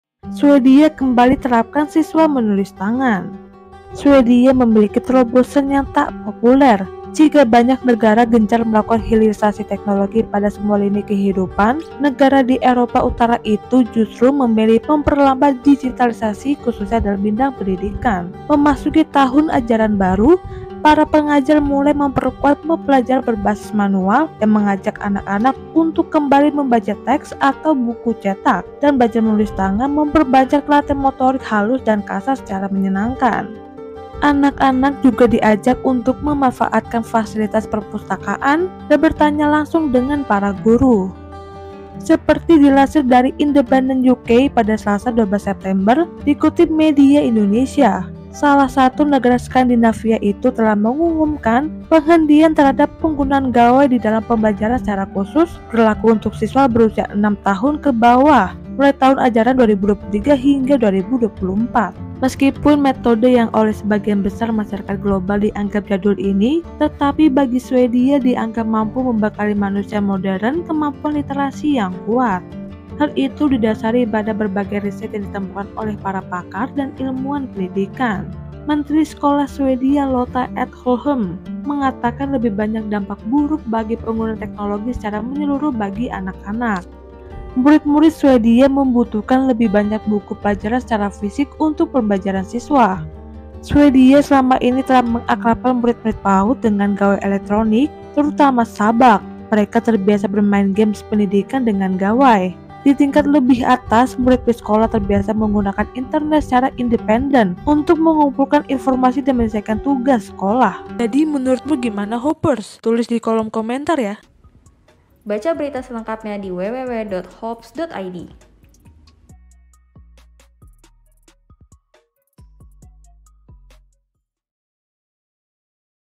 HINDARI PEMAKAIAN GADGET, SEKOLAH DI SWEDIA KEMBALI TERAPKAN MENULIS TANGAN
SWEDIA memiliki terobosan yang tak populer. Jika banyak negara gencar melakukan hilirisasi teknologi pada semua lini kehidupan, negara di Eropa Utara itu justru memilih memperlambat digitalisasi khususnya dalam bidang pendidikan.

Menteri Sekolah Swedia Lotta Edholm mengatakan lebih banyak dampak buruk dari penggunaan teknologi secara menyeluruh bagi anak-anak.